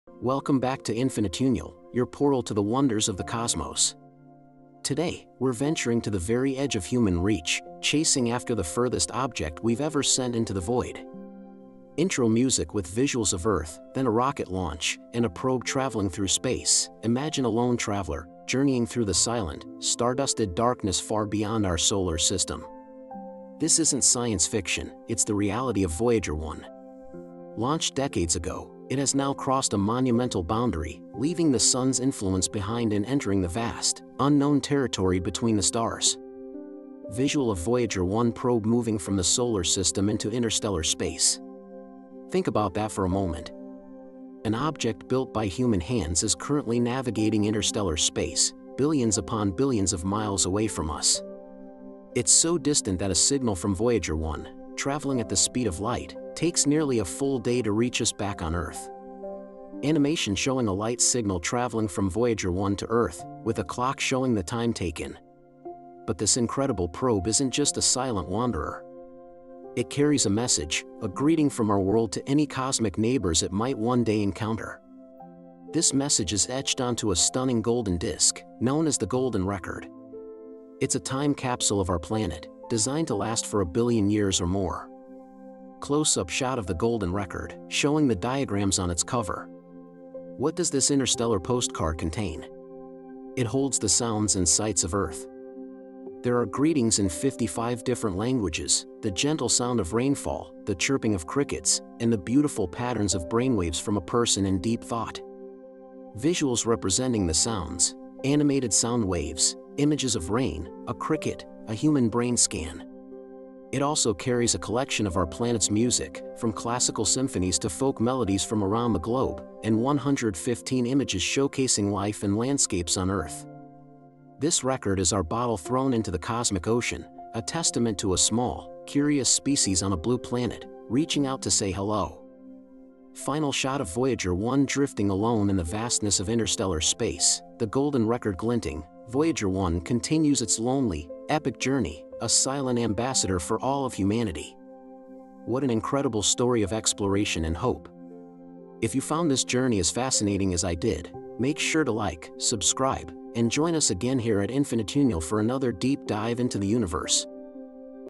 The Furthest Man-Made Object 🛰️
Voyager 1 is billions of miles away, in interstellar space. It carries a Golden Record with sounds of Earth, in case aliens ever find it.